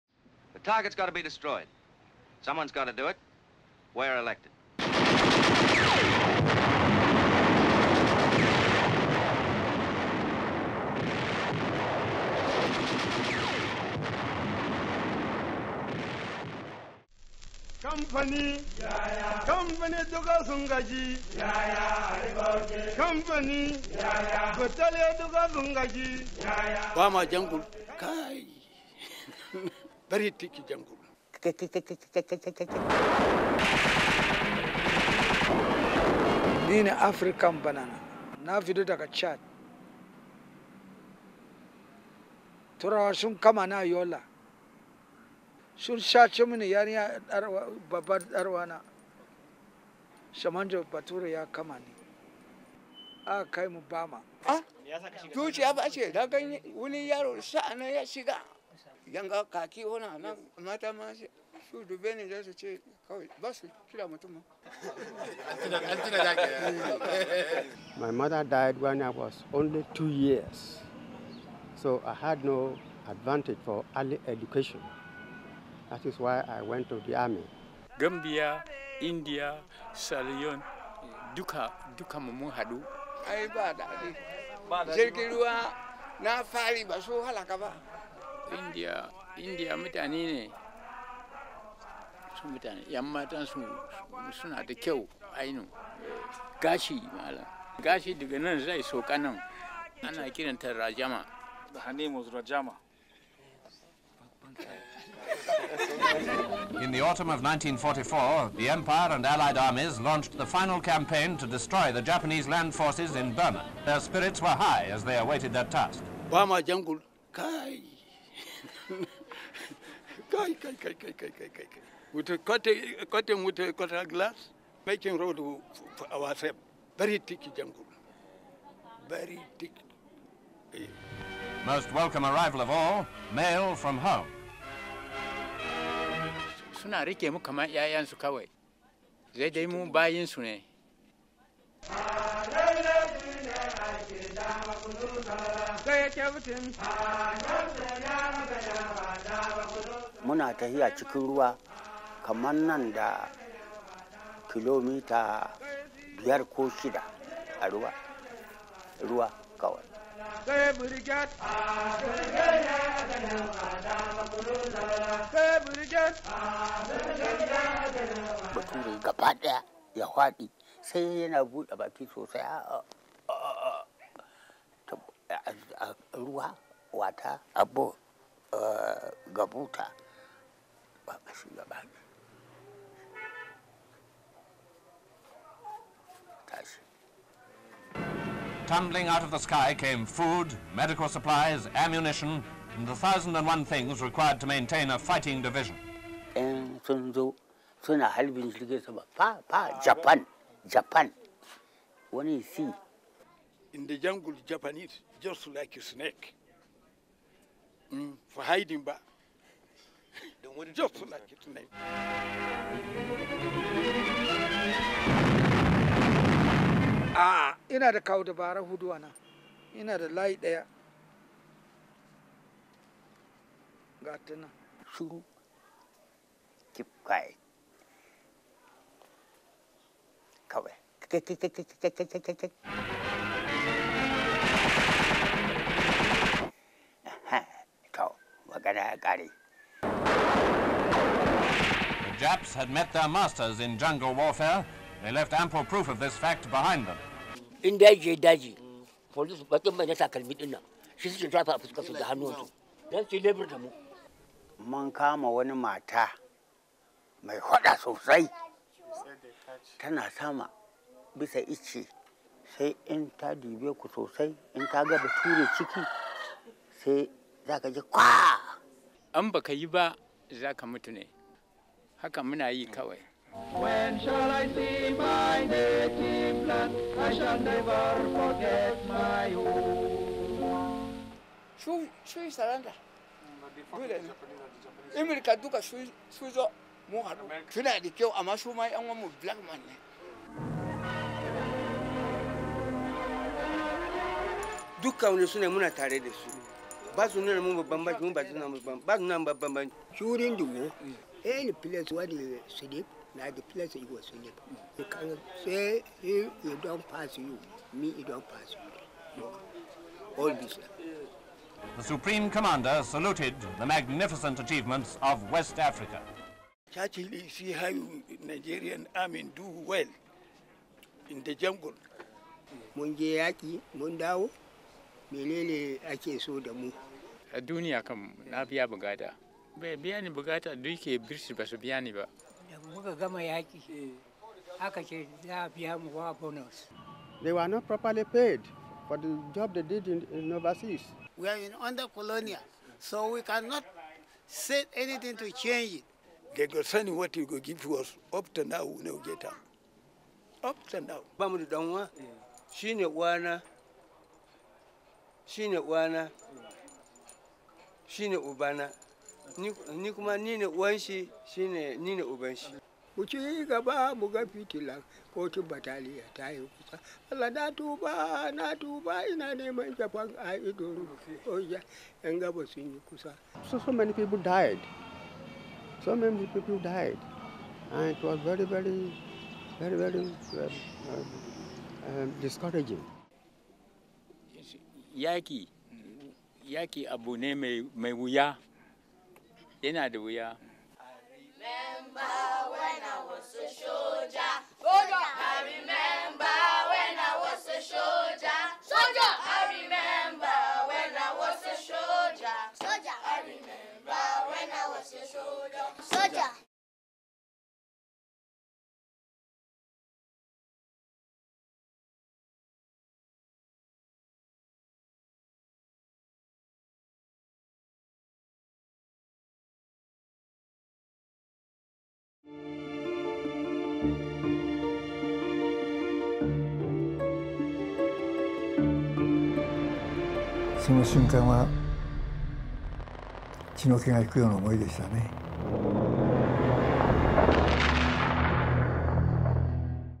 WWII's forgotten army: West Africa's soldiers in Burma | Guardian Features
Nigerians made up more than half of the total force of 90,000 West African soldiers deployed to south east Asia after 1943, as part of the British army’s 81st and 82nd (West Africa) Divisions.
But while the role of Indians and Gurkhas in the campaign to drive the Japanese out of Burma is well-known, allied commander General William Slim did not mention the African soldiers in his speech thanking the 14th army.

Seventy years on, many remain bitter that their contribution was never recognised fully. This is their story, of surviving two years of intense jungle warfare, of helping secure Victory over Japan, through exclusive, never before seen footage of West Africa divisions fighting in Burma, courtesy of the Imperial War Museum, and Royal Commonwealth Ex-Services League.